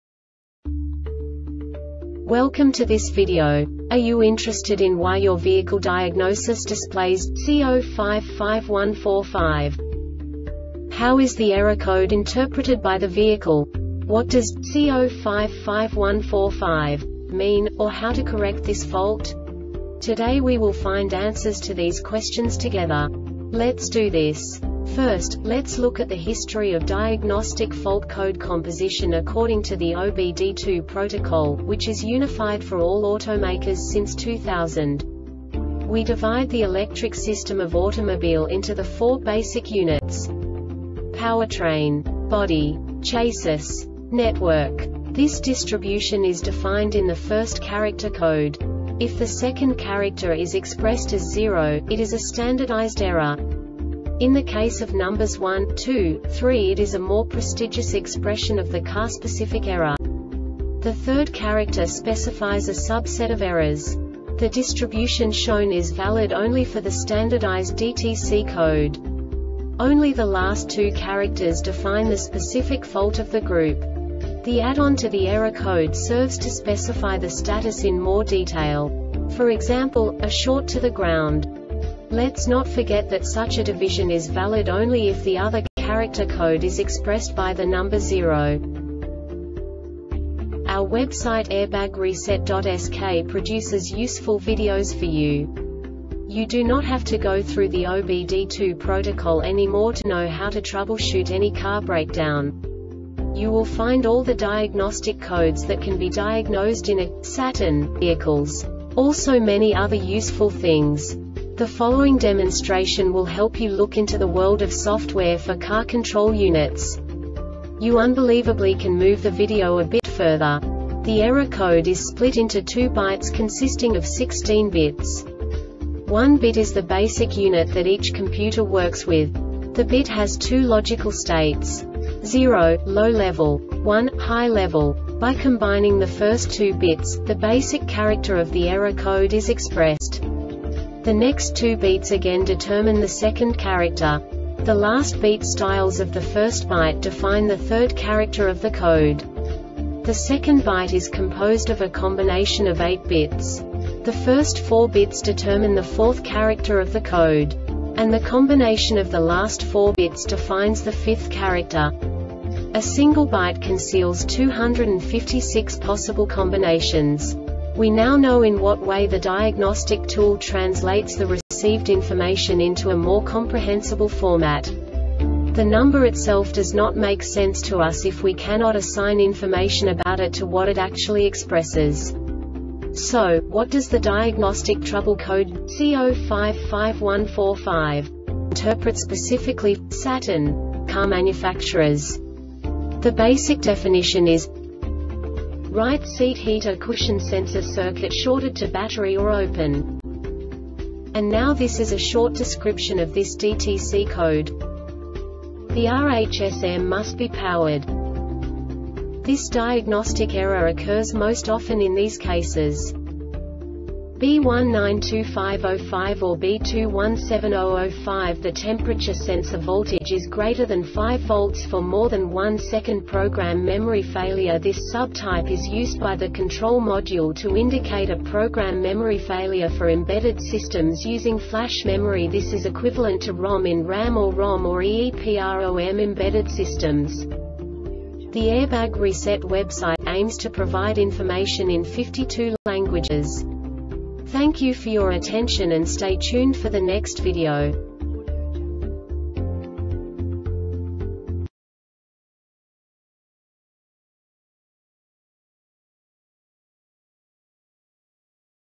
DTC Saturn C0551-45 Short Explanation
Contents:
0:21 Basic DTC analysis according to OBD2 protocol standard.
1:48 Insight into programming
2:58 A diagnostically understandable description of the Diagnostic Trouble Code
3:05 Basic error definition

Make:
Saturn
Code:
C0551 45
Definition:
Right Seat Heater Cushion Sensor Circuit Shorted to Battery or Open
Description:
The RHSM must be powered.
Cause:
B1925 05 or B2170 05The temperature sensor voltage is greater than 5 V for more than 1 second.
Failure Type:
Program Memory Failure
This sub type is used by the control module to indicate a program memory failure for embedded systems using FLASH memory. This is equivalent to ROM in RAM or ROM or EEPROM embedded systems.